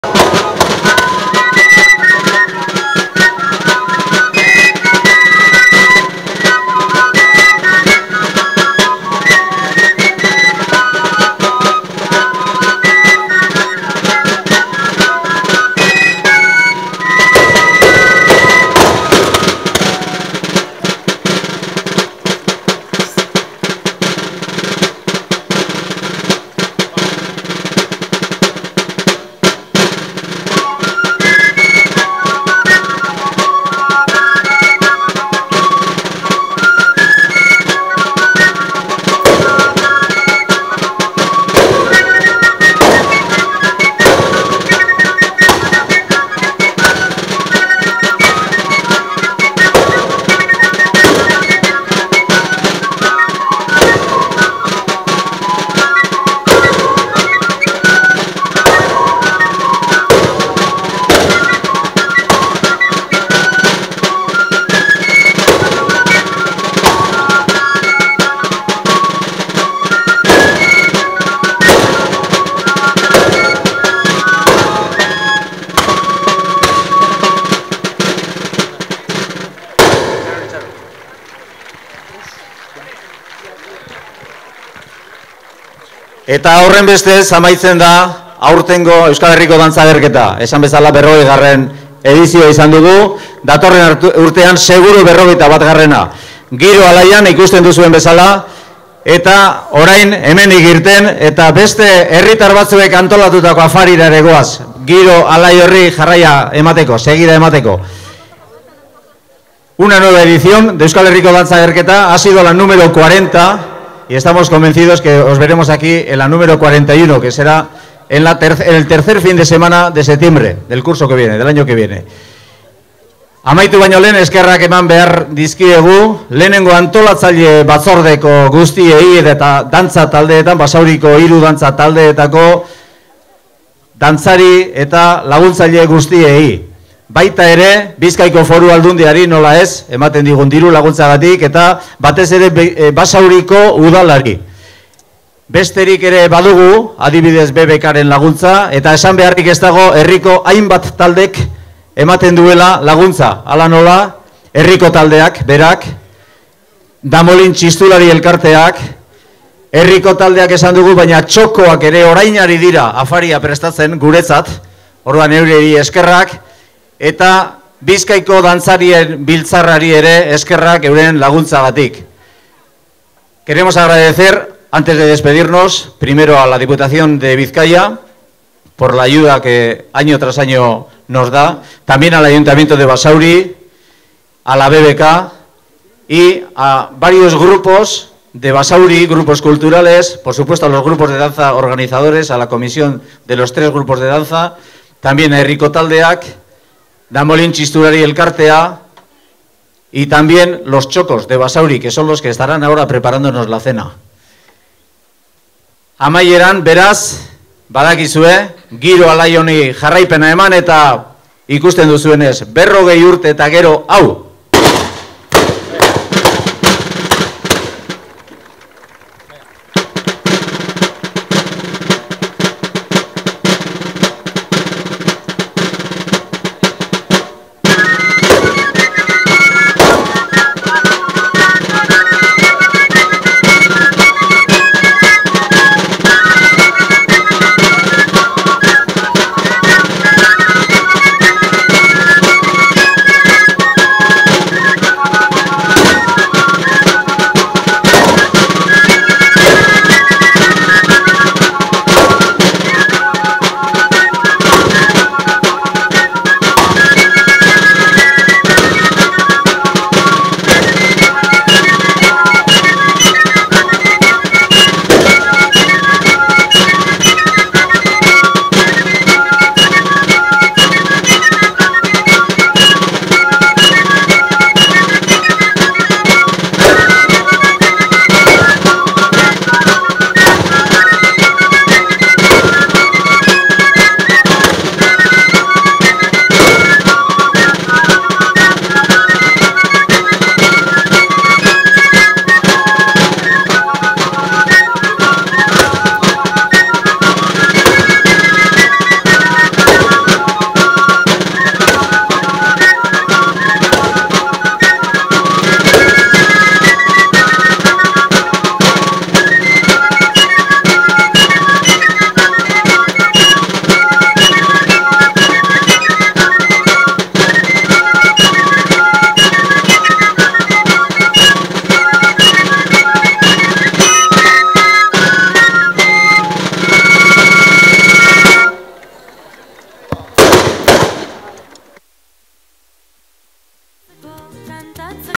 201209150701 Euskal Herriko 40. dantza agerketan parte hartu zuten guziek dantzan, 2012an Basaurin
Pantaila osoan ikusi liteke bideoa. Informazioa
2012ko irailaren 15ean Basaurin ospatu zen Dantza Agerketaren amaieran, parte hartu zuten guztiek elkarrekin dantzatu zuten Jota eta Arin-arina.

Dantzariak: Elorrioko Errebonbiloak, Otsagiko dantzariak, Itsassu-Lapurdiko Ataitze dantza taldea, Zumarragako Irrintzi dantza taldea, Laguardia - Biasteriko dantza taldea eta Xiberoko dantzariak.
Musikariak: Zumarragako Antxinako Ama, Txistulari Taldea
Bideoaren zear entzuten diren tiro hotsak, Elorrioko Errebonbiloarenak dira.
Ref. 22556